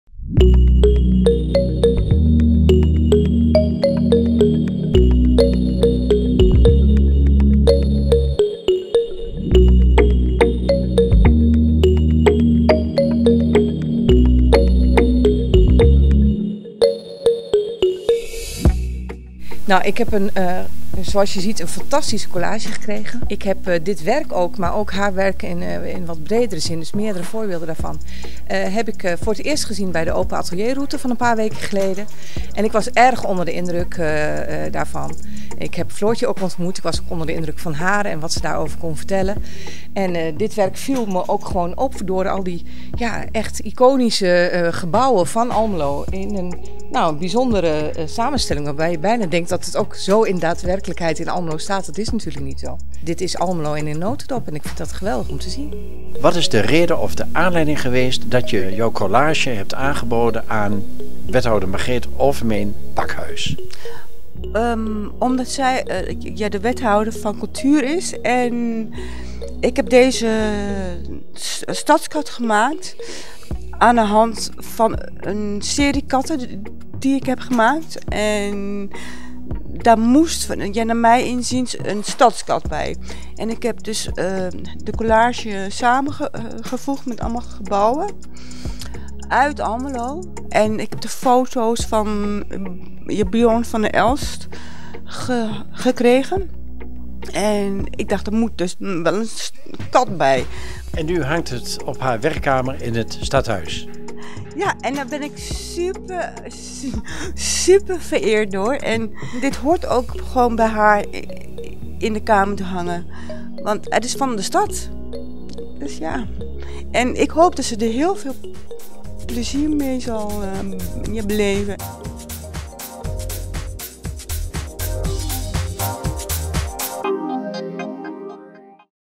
Overhandiging Kunstwerk aan Wethouder
De Almelose stadskat die Floortje en Dennis overhandigden aan wethouder Overmeen-Bakhuis, is een spinnende kater te midden van alle herkenbare Almelose gebouwen.